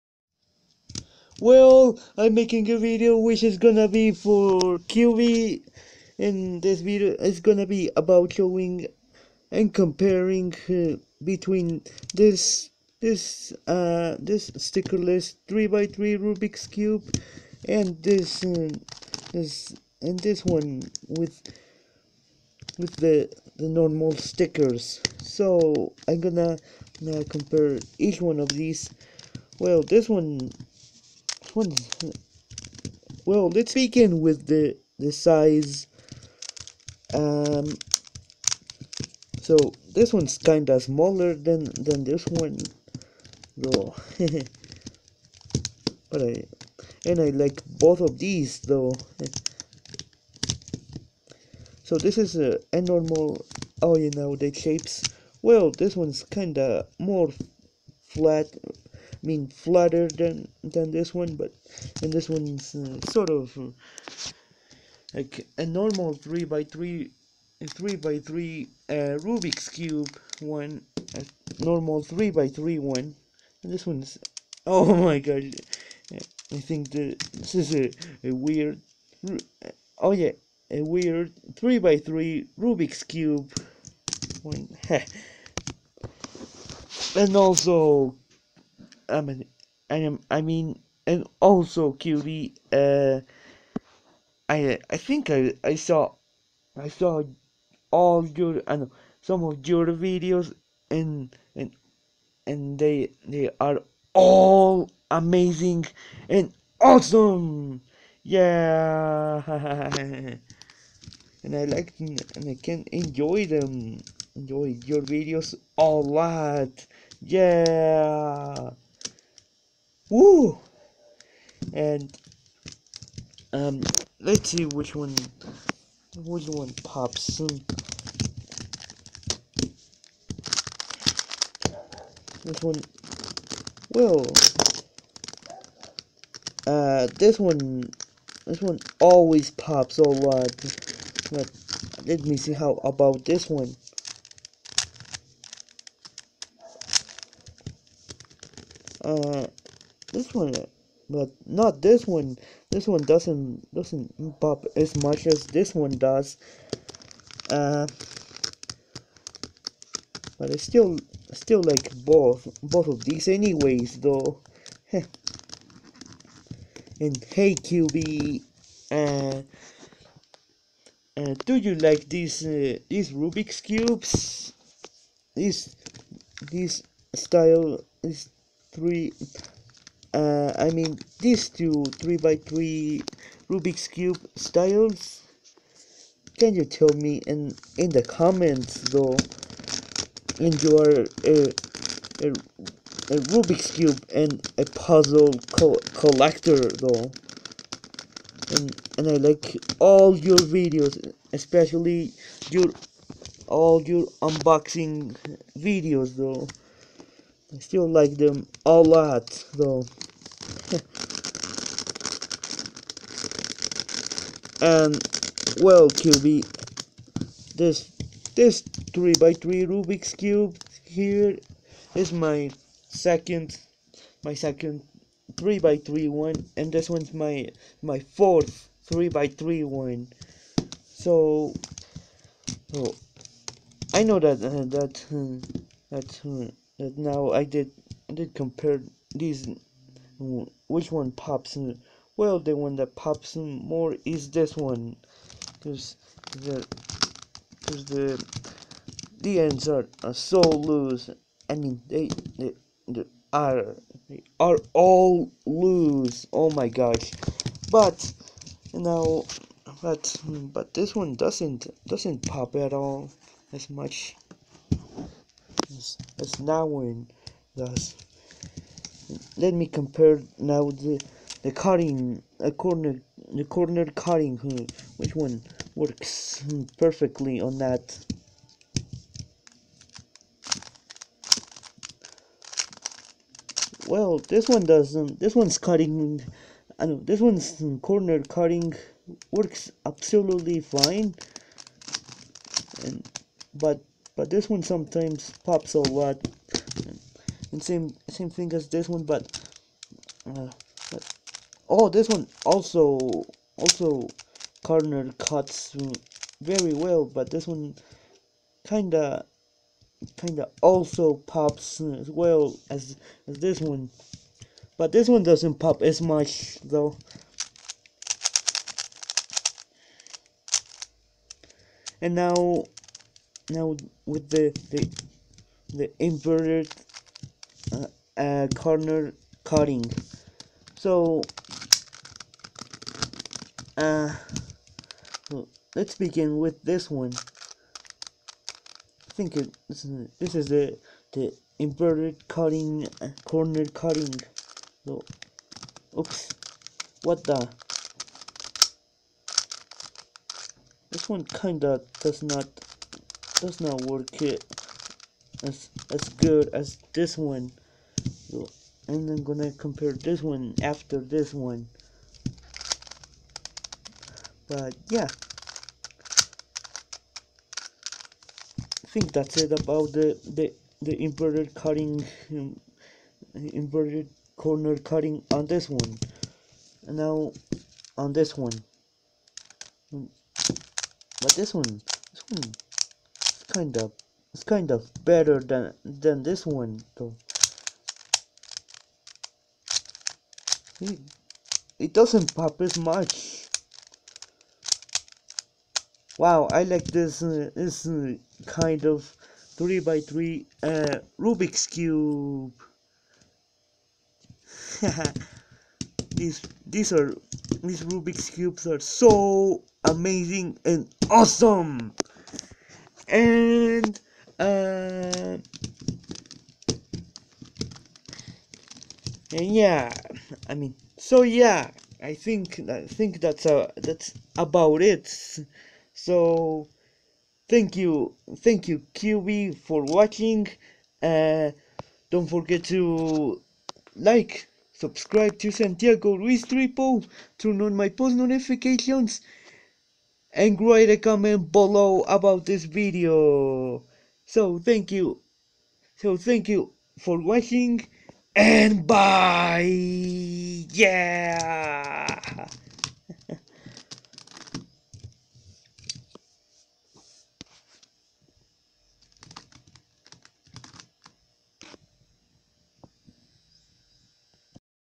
For Cuby!!!
Hi Cuby!!!

Well, on this video I will show you and compare 2 of my 3x3 rubik´s cubes that I´m collecting right now!!!

An also, Cuby all your videos are very very very very very very very very awesome!!!